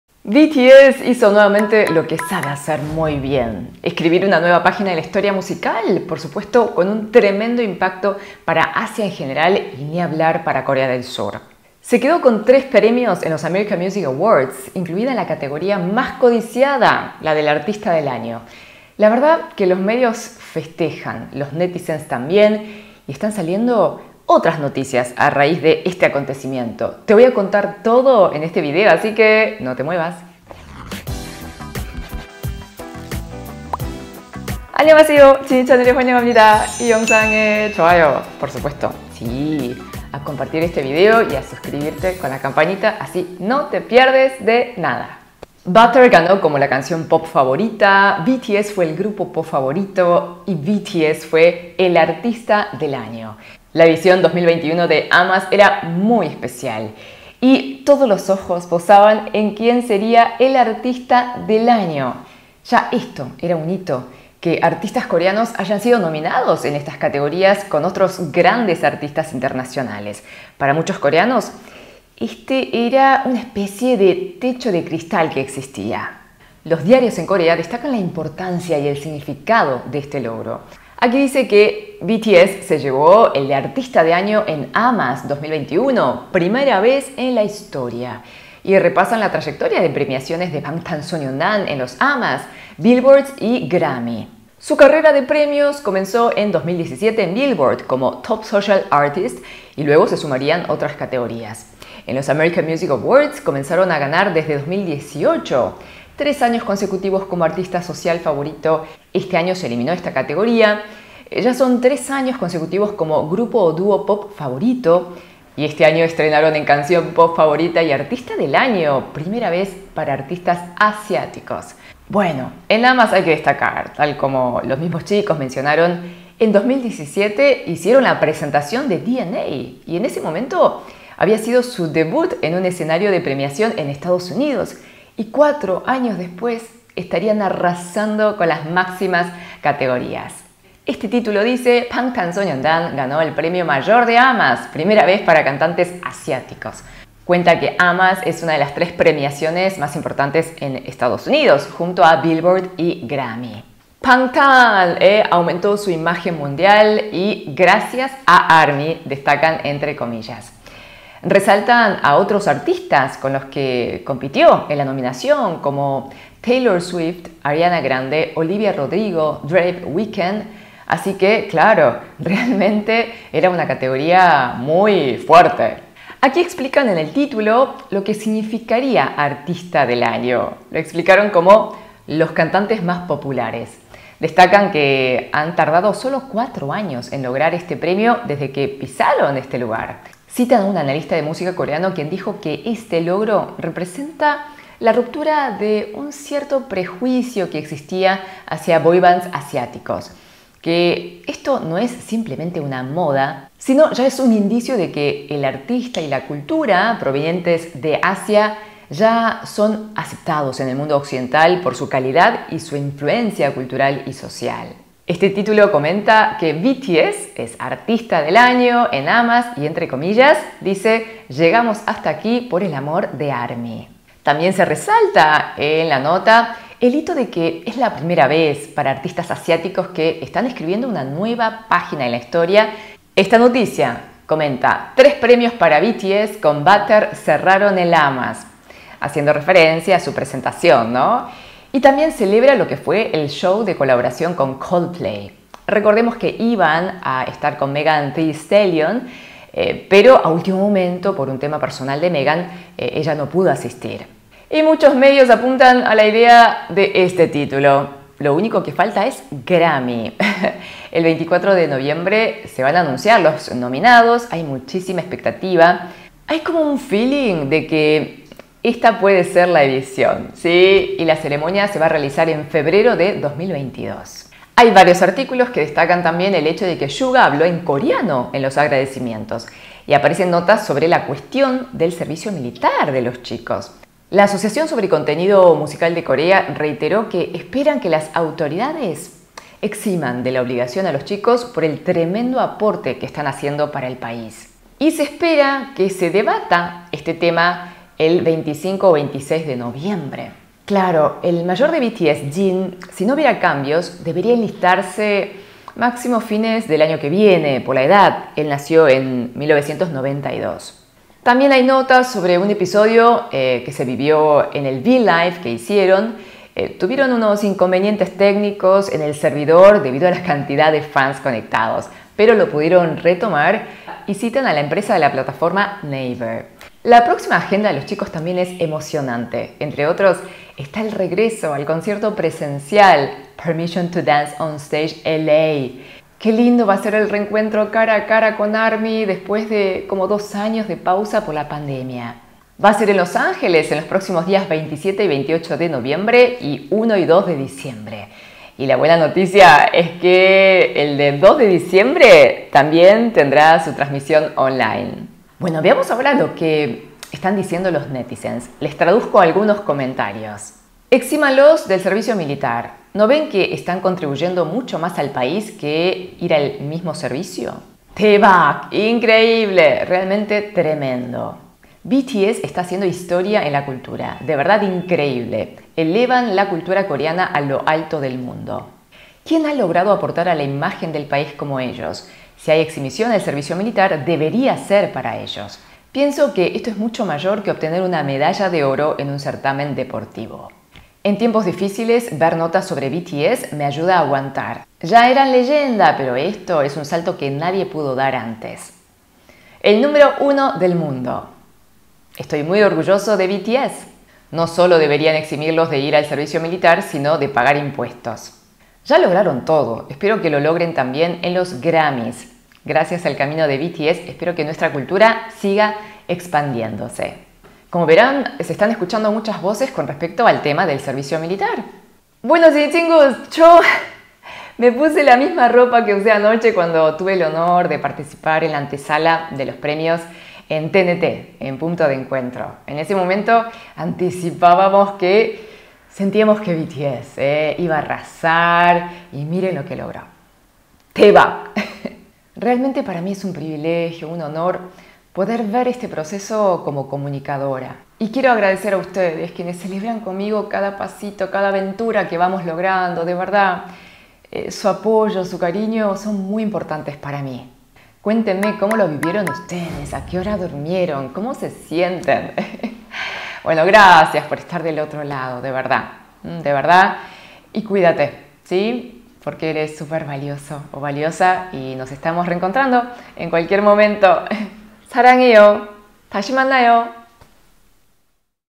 🎉¡BTS “ARTISTA DEL AÑO” EN LOS AMAS 2021! PRIMER GRUPO ASIÁTICO EN RECIBIR EL GALARDÓN👏
Anñong mis valiosos JiniChingus  💞 BTS vuelve a hacer historia al obtener 3 premios en los American Music Awards, entre ellos, la categoría codiciada de Artista del Año.  Te cuento todas las repercusiones en Corea.

✔ JiniTrend Instagram (KBeauty y KFashion)